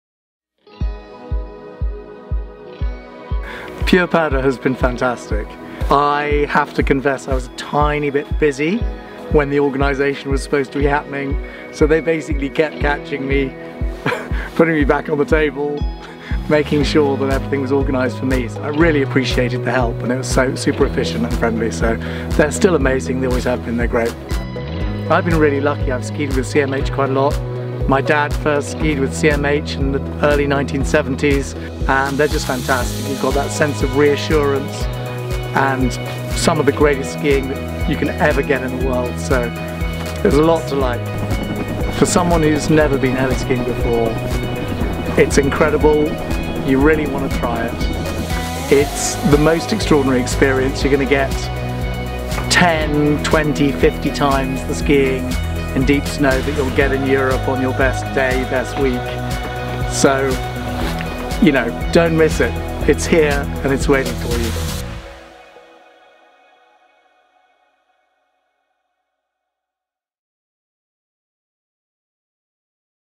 Pure Powder Testimonial - Patrick (CMH Heli Skiing)
Don't take our word for it - feedback from one of our regular Pure Powder customers.  Filmed on location at the CMH Heli Skiing Cariboos Lodge.